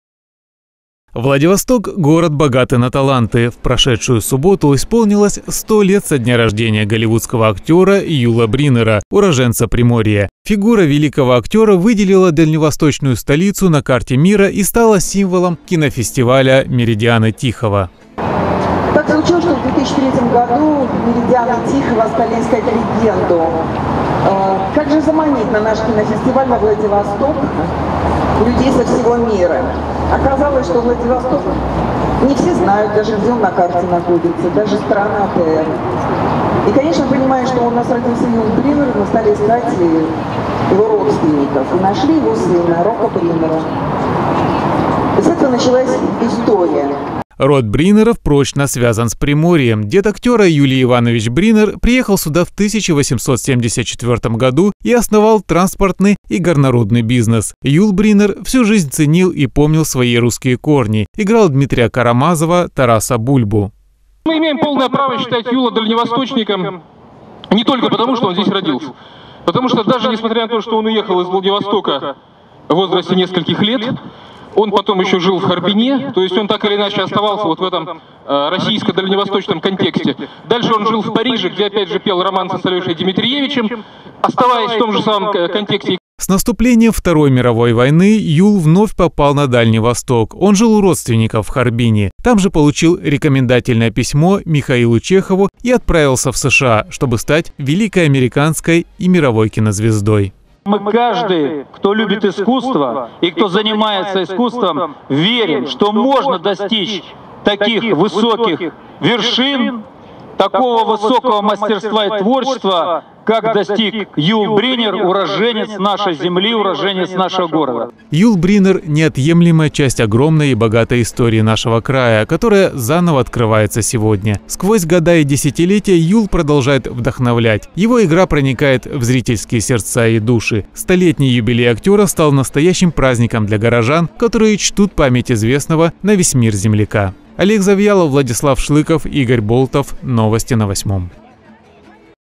Бриннеру - 100!
Род Бринеров прочно связан с Приморьем. Дед актёра, Юлий Иванович Бринер, приехал сюда в 1874 году из основал, транспортный и горнорудный бизнес. Юл Бриннер всю жизнь ценил и помнил свои русские корни. Играл Дмитрия Карамазова, Тараса Бульбу.